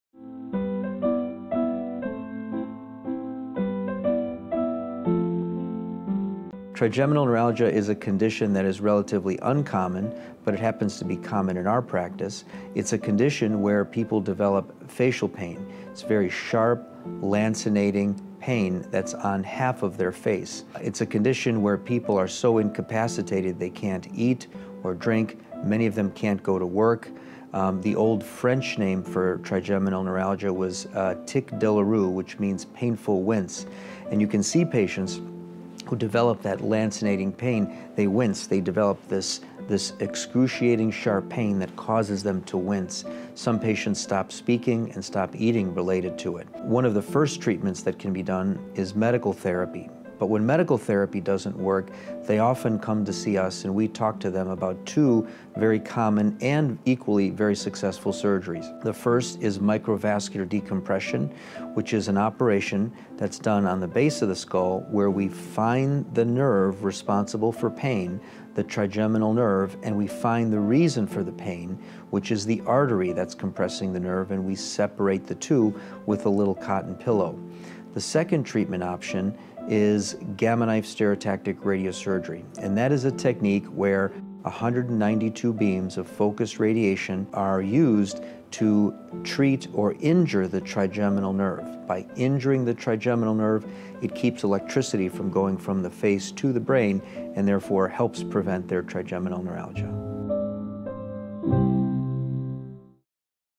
Trigeminal Neuralgia, Explained — AMITA Health
George Bovis, MD, an AMITA Health neurosurgeon based in Chicago’s northwestern suburbs, explains the uncommon condition known as trigeminal neuralgia, which is characterized by a sharp, lancing pain on one half of the face (hence its original name in French, tic douloureux (painful wince)). This pain can be so incapacitating that sufferers can barely eat, speak, drink or go to work.

Dr. Bovis typically begins treatment with medical therapy. If therapy is unsuccessful, he recommends one of two equally effective treatments:

1. Microvascular decompression, which uses a cotton pillow to keep the artery from compressing the trigeminal nerve at the base of your skull
2. Gamma Knife stereotactic radiosurgery, which focuses 192 beams of radiation at the trigeminal nerve, preventing it from transmitting pain signals to the face from the brain